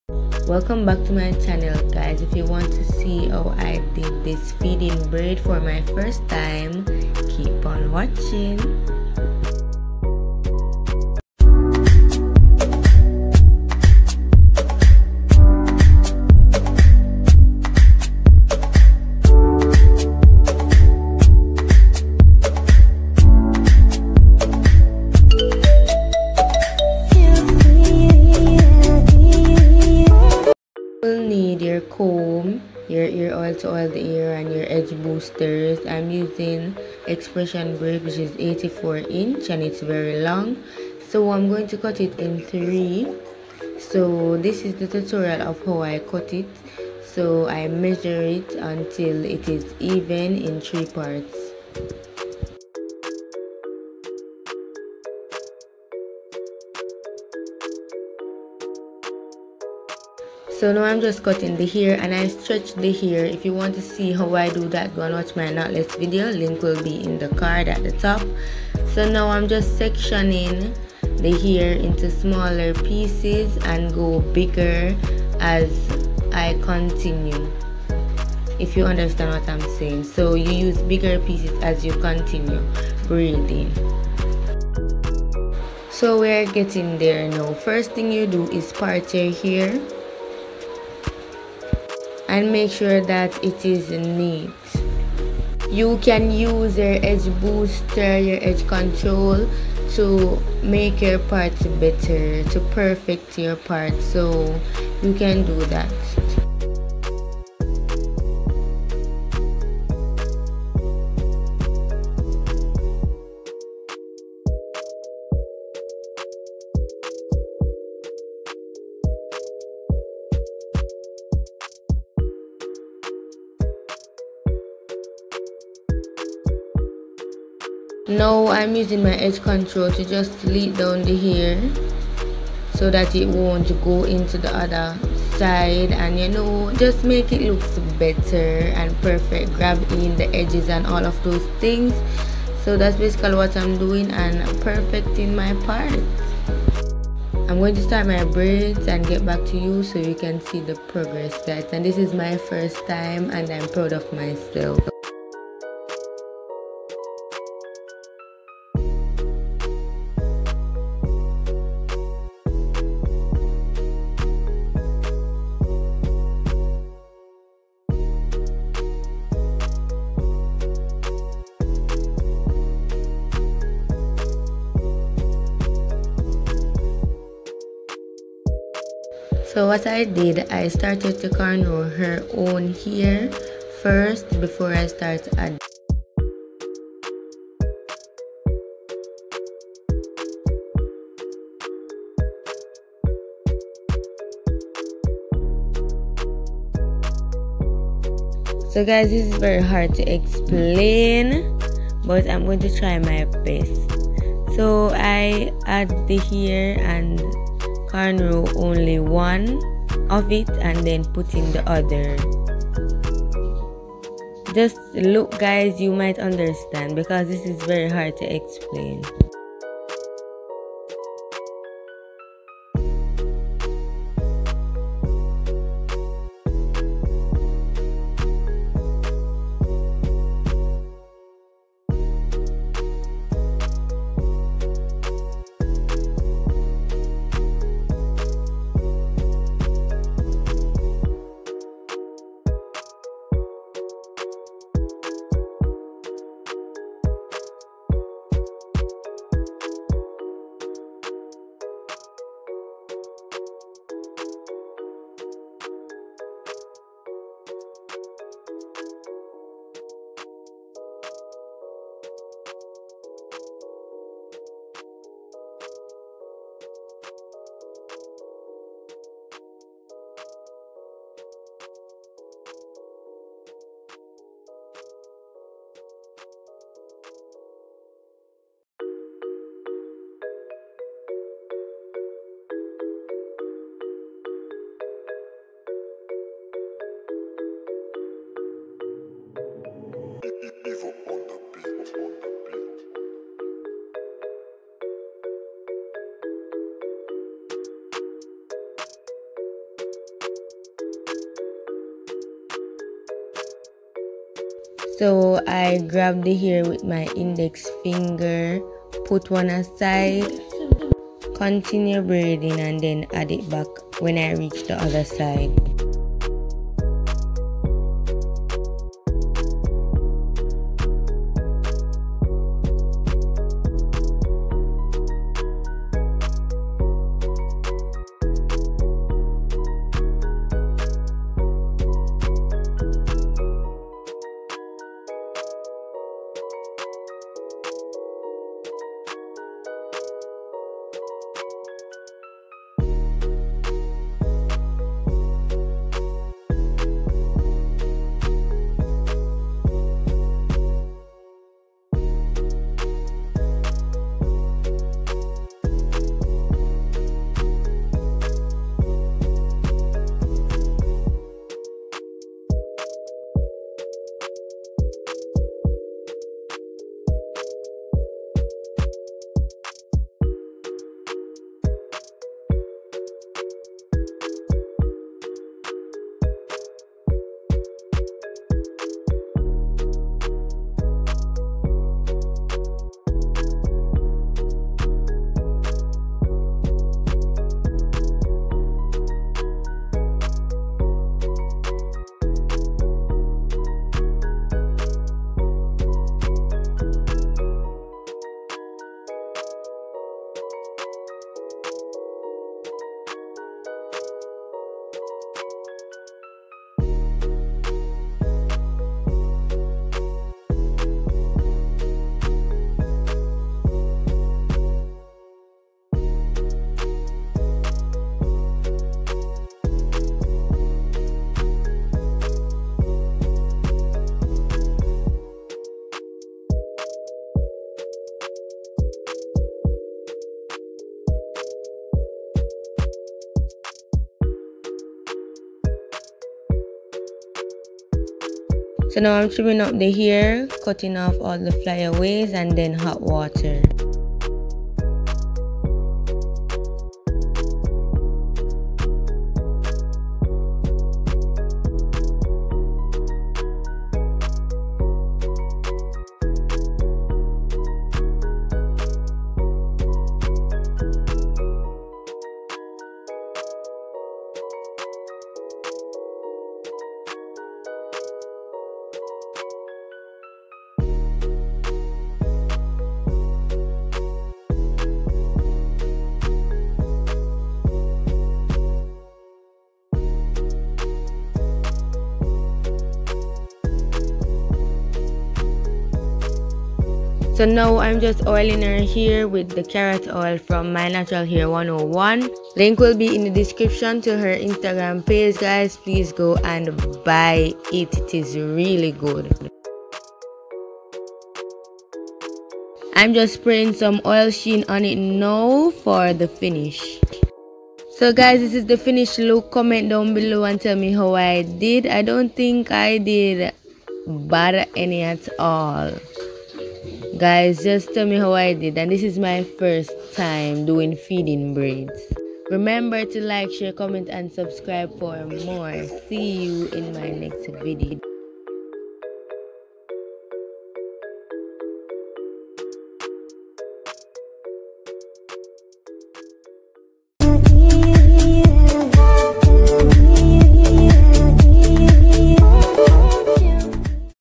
How To Do Feed-in Braids• Easy Technique for Beginners|| My first time doing cornrow braids
Welcome back "waa gwaan fren" I'm back with another hair video... first time trying to do feed-in braid and it was a success what do you think?